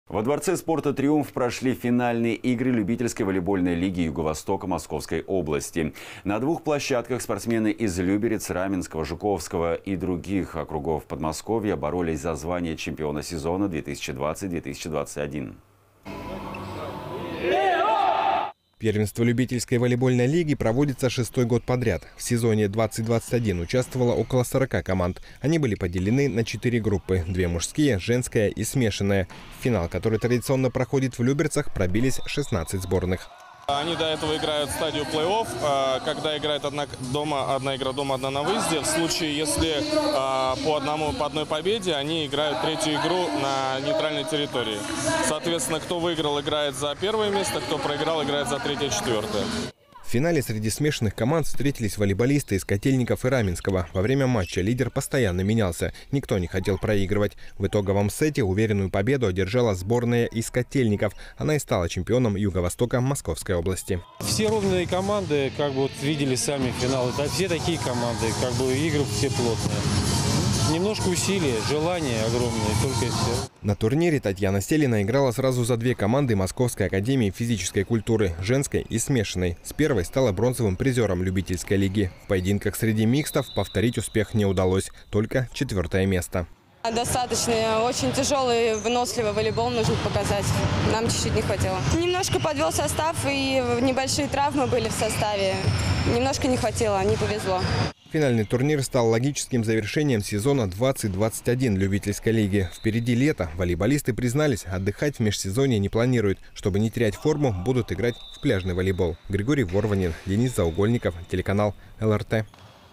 Любительская лига по волейболу завершила сезон
Во дворце спорта «Триумф» прошли финальные игры Любительской волейбольной лиги юго-востока Московской области. На двух площадках спортсмены из Люберец, Раменского, Жуковского и других округов Подмосковья боролись за звание чемпиона сезона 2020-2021.

Первенство любительской волейбольной лиги проводится 6 год подряд. В этом году участвовало около 40 команд. Они были поделены на 4 группы: две мужские, женская и смешанная. В финал, который традиционно проходит в Люберцах, пробились 16 сборных.

Владимир Майоров, председатель Федерации волейбола г.о. Люберцы:
«Они до этого играют стадию плей-офф, когда играют одну игру дома одну на выезде. В случае если по одной победе, они играют одну игру на нейтральной территории. Соответственно, кто выиграл, играют за первое место, кто проиграл за третье-четвертое».

В финале среди смешанных команд встретились волейболисты из Котельников и Раменского. Во время матча лидер постоянно менялся. Никто не хотел проигрывать. В итоговом сете уверенную победу одержала сборная из Котельников. Она и стала чемпионом юго-востока Московской области.

Алексей Сельдемиров, чемпион Любительской волейбольной лиги:
«Все ровные команды, сами видели финал. Игры все плотные. Немножко усилия, желания только и всего».

На турнире Татьяна Селина играла сразу за две команды Московской академии физической культуры – женской и смешанной. С первой стала бронзовым призером любительской лиги. В поединках среди микстов повторить успех не удалось, только 4 место.

Татьяна Селина, бронзовый призер Любительской волейбольной лиги:
«Достаточно выносливый волейбол нужно показать нам чуть-чуть не хватило. Немножко подвел состав. Небольшие травмы были в составе. Не повезло».

Финальный турнир стал логическим завершением сезона 20-21 любительской лиги. Впереди лето. Волейболисты признались, отдыхать в межсезонье не планируют. Чтобы не терять форму будут играть в пляжный волейбол.